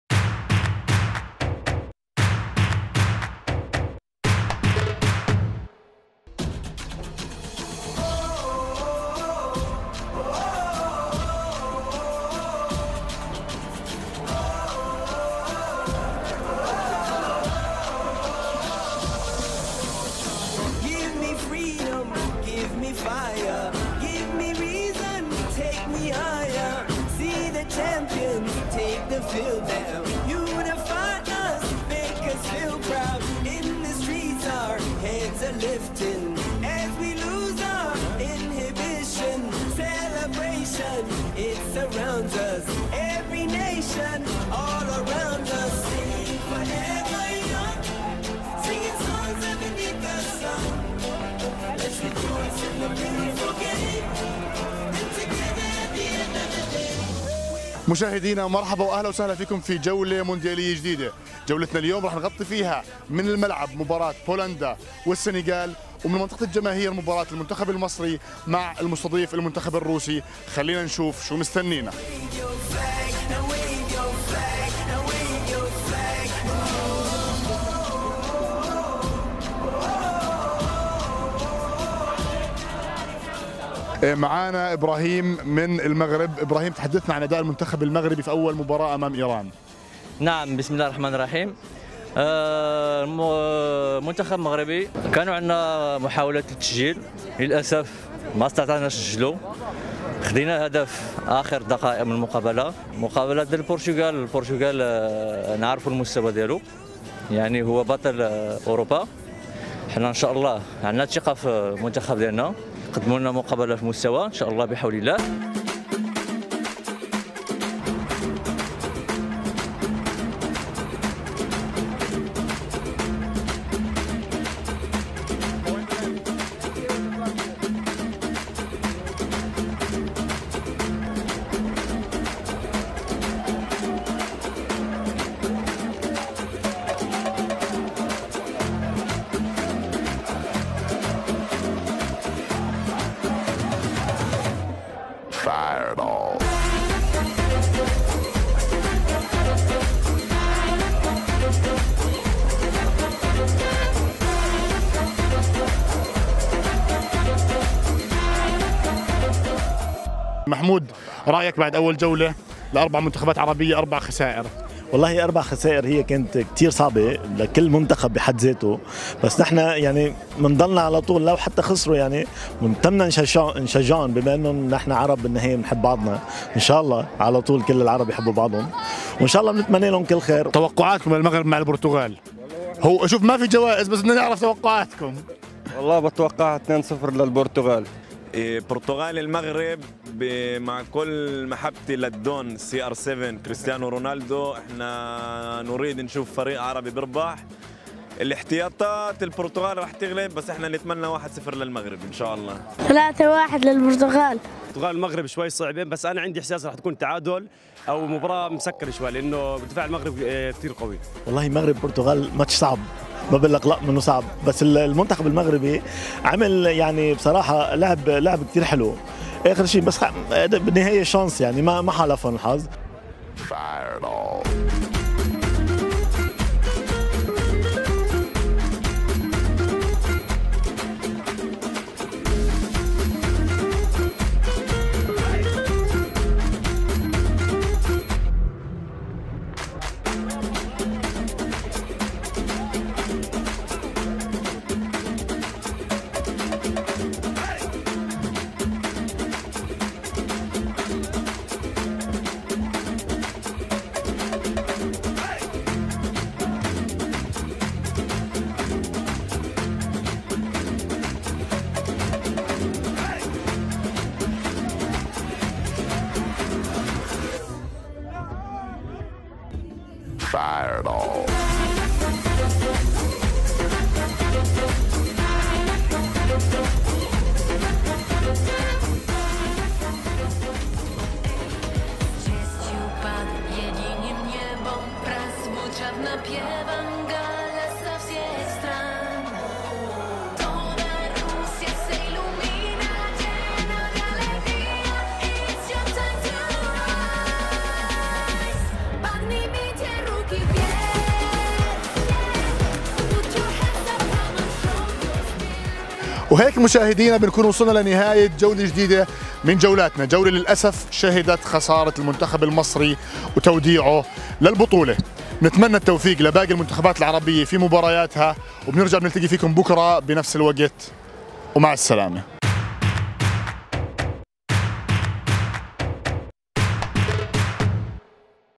اجواء اليوم السادس من كأس العالم - تقرير من روسيا
تقرير يومي للزميل محمد الحمايدة من روسيا يطلعنا فيه على اخر اخبار المونديال وتفاعل الجماهير ومقابلات معهم خلال البطولة.